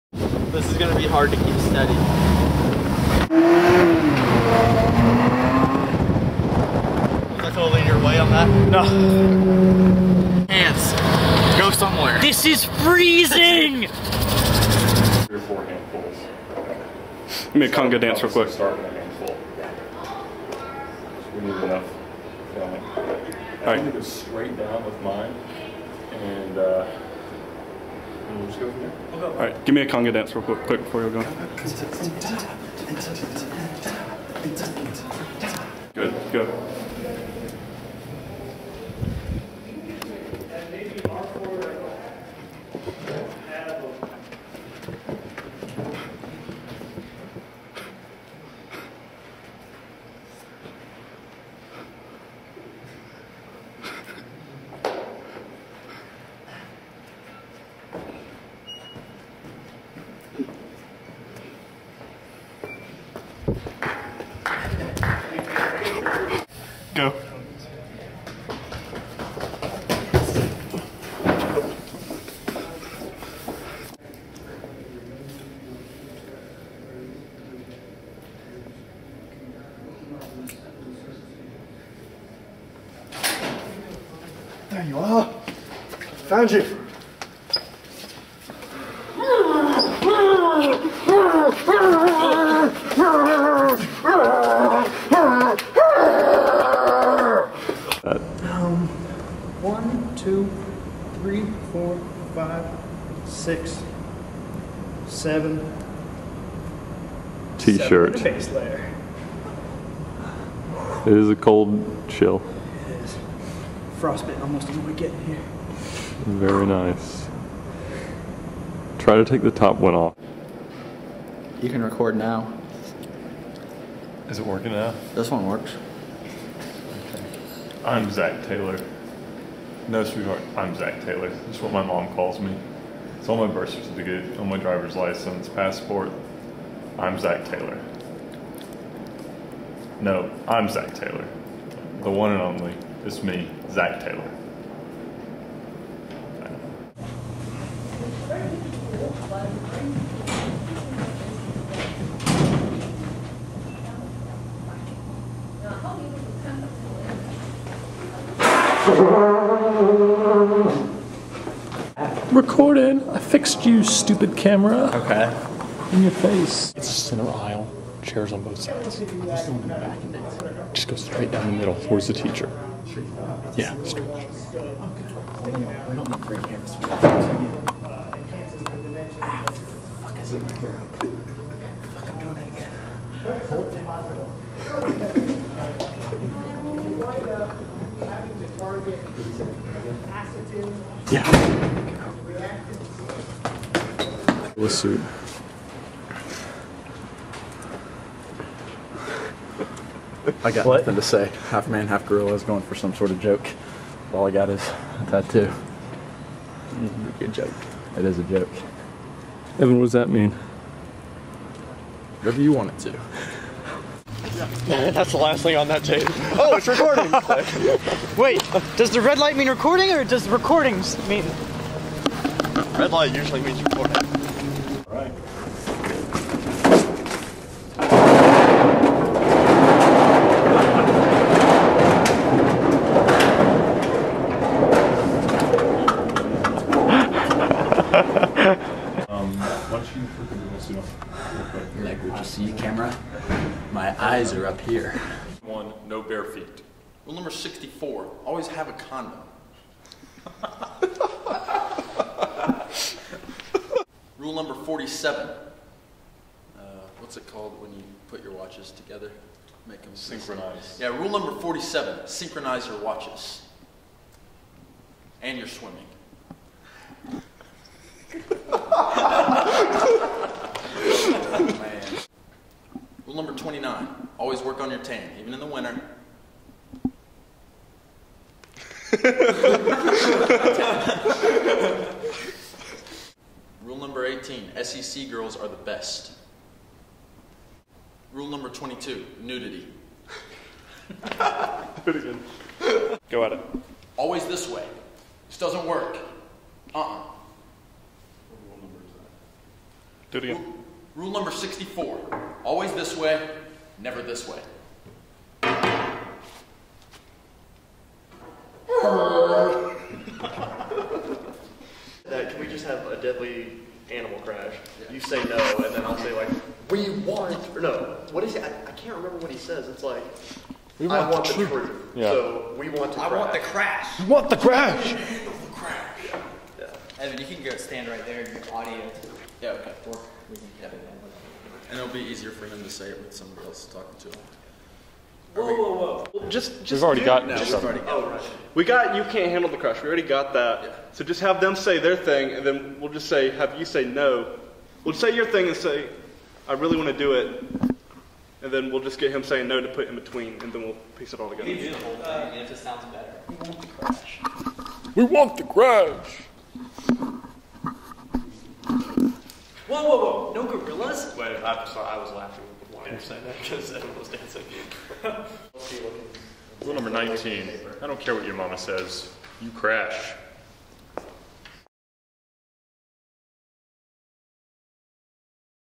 Lecture Crashers 4: Behind the Scenes and Bloopers
Here's a look at the some behind the scenes footage and out-takes from LC4.